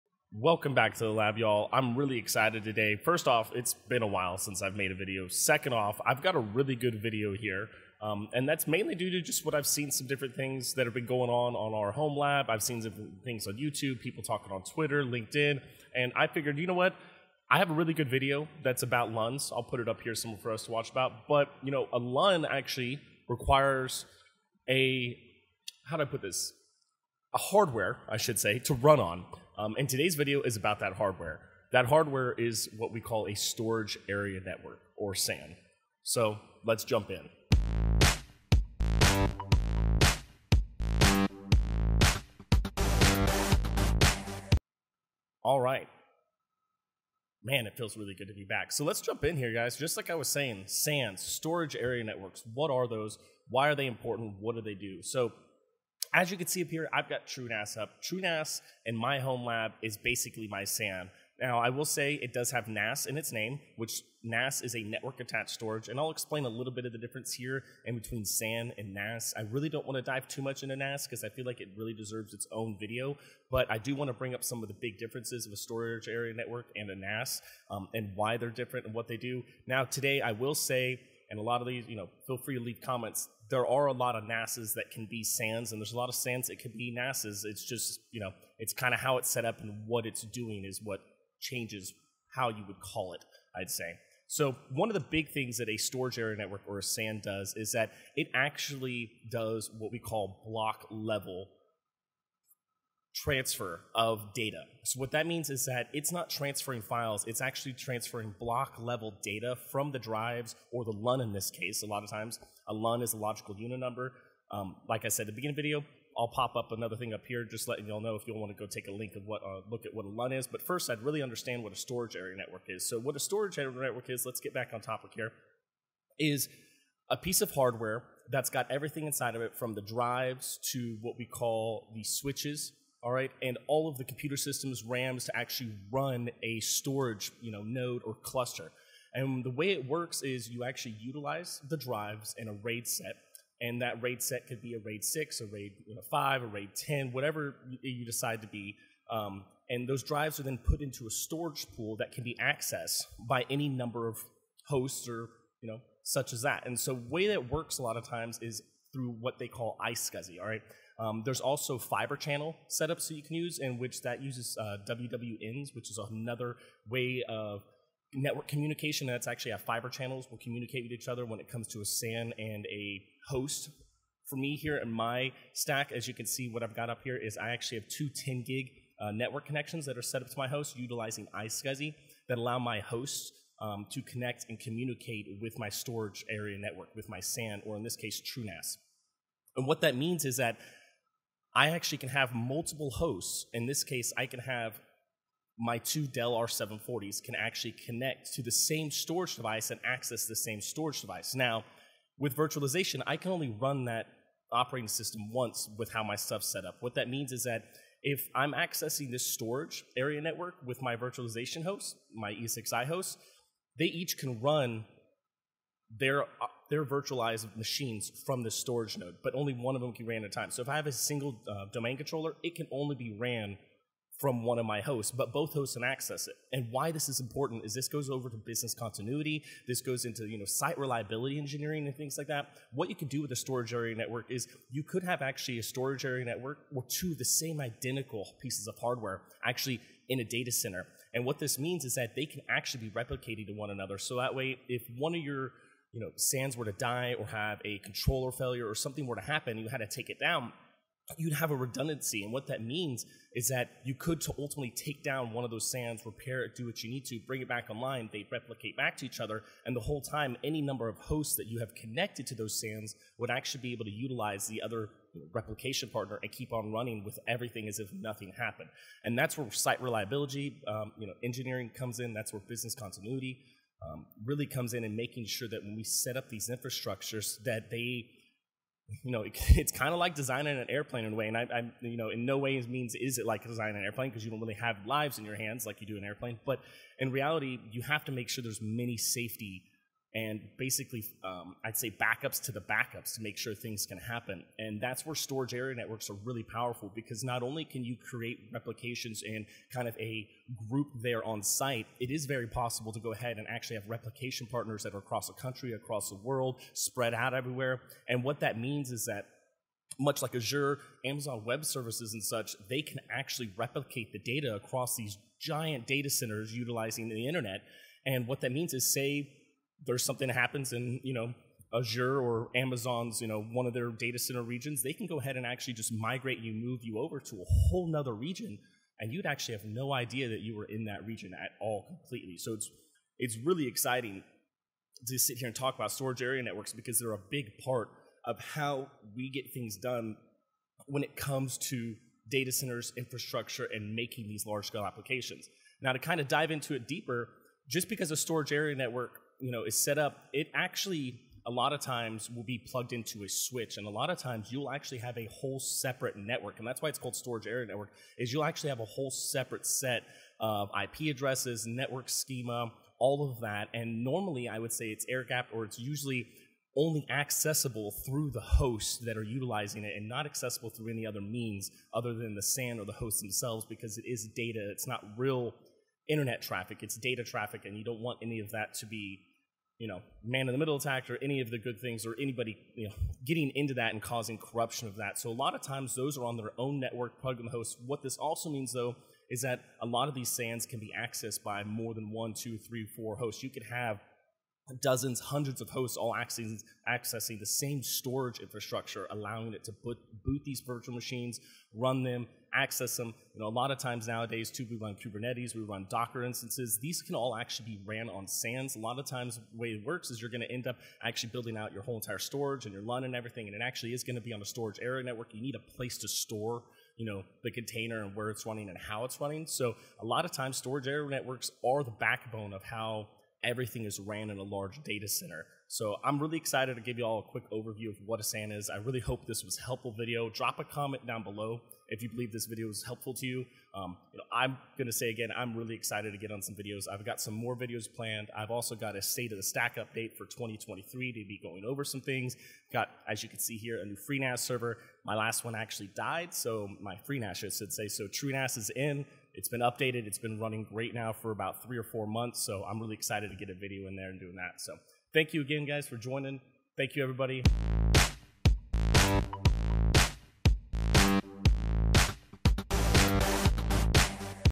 Enterprise Storage Basics: What is a SAN?
In this Basics Video, I review what a SAN is and why it's essential to Enterprise infrastructure.